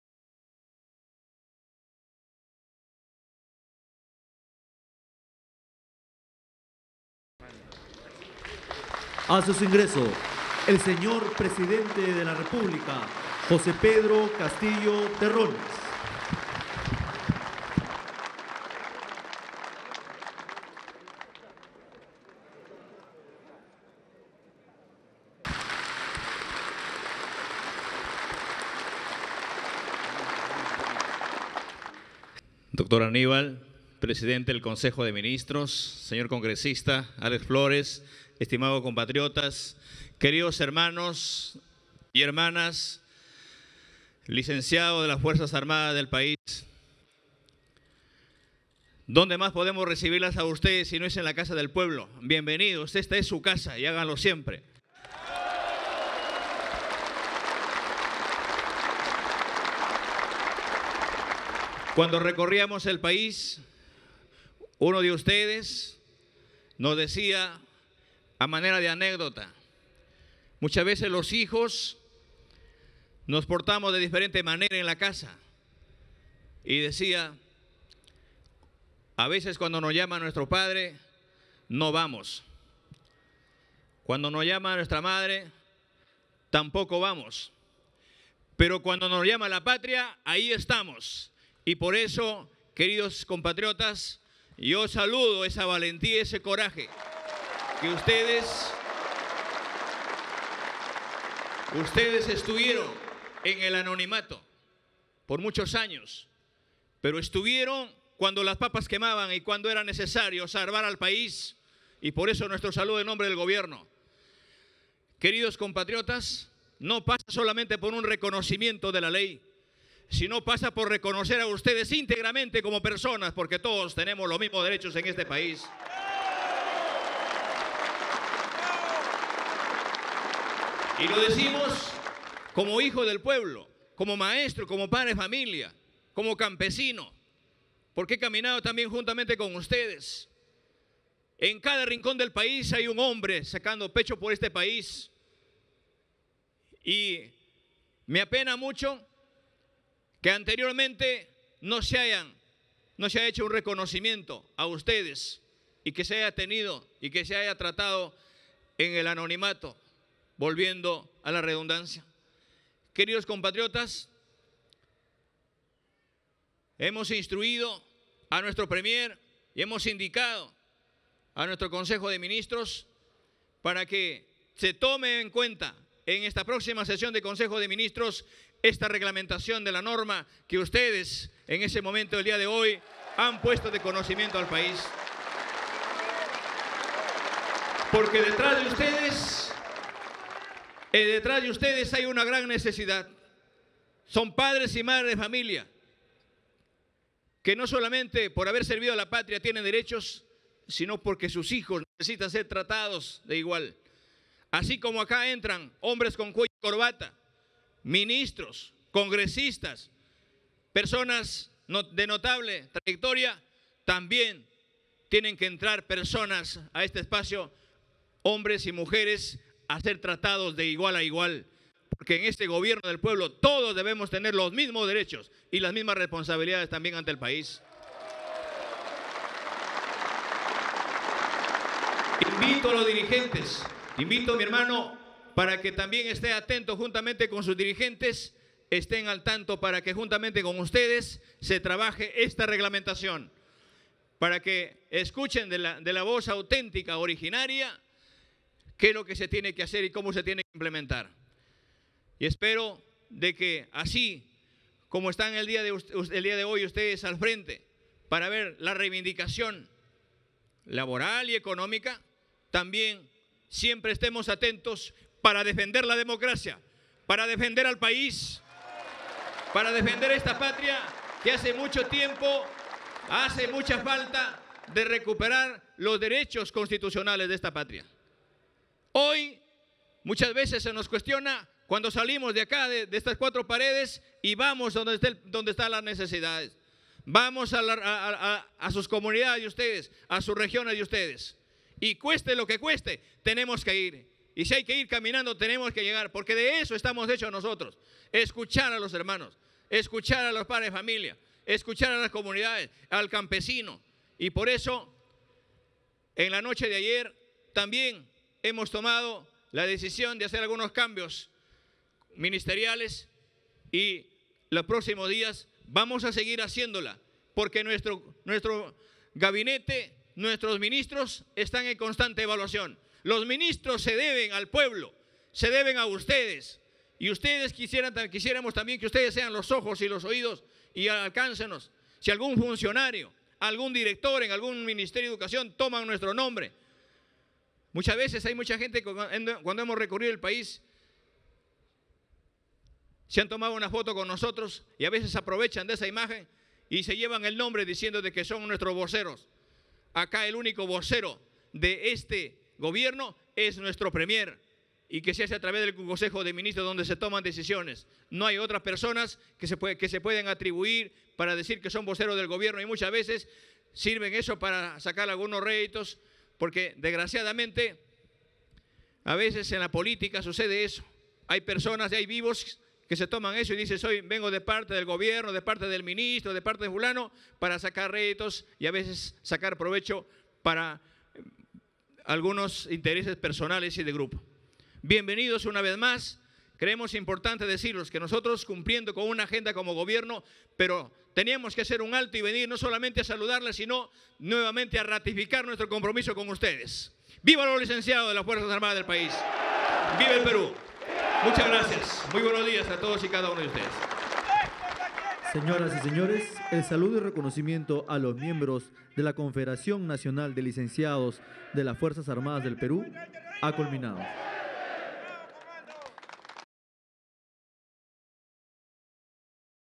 Presidente Pedro Castillo recibe a licenciados de las Fuerzas Armadas
El presidente Pedro Castillo recibió a licenciados de las Fuerzas Armadas en Palacio de Gobierno.